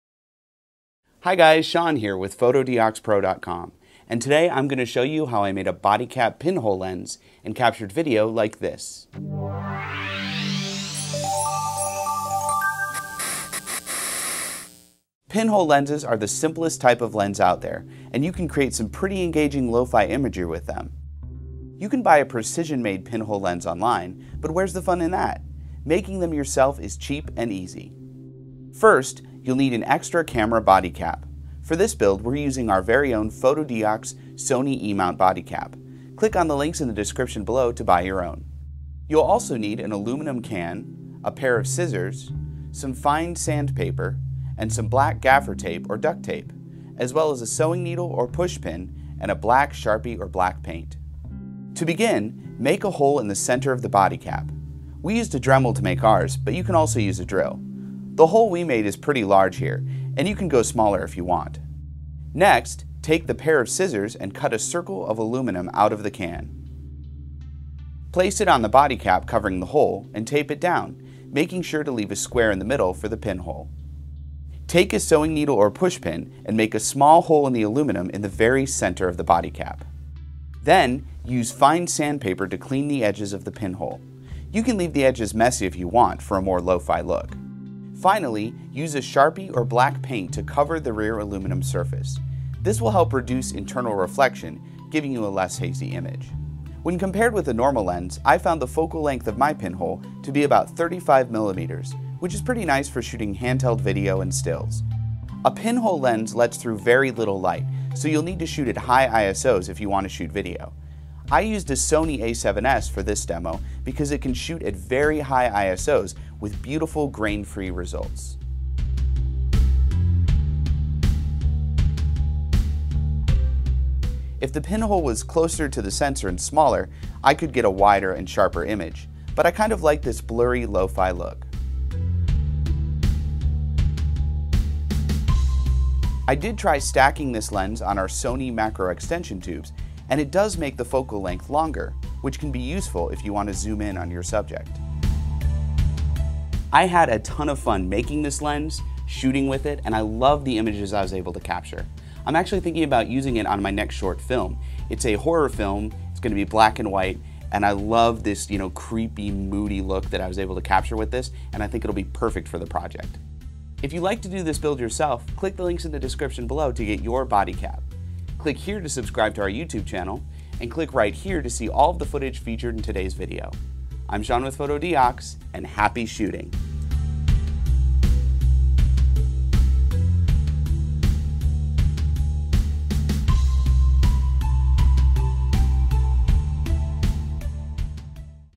How to make a Body Cap Pinhole Lens for your Digital Camera
Making a body cap pinhole lens for your digital camera is one of the cheapest and easiest DIY photography projects out there, and in only a couple minutes you'll have a homemade "lens" that captures surreal lo-fi photos and videos. Check out our tutorial video showing you how to make a DIY pinhole lens of our own.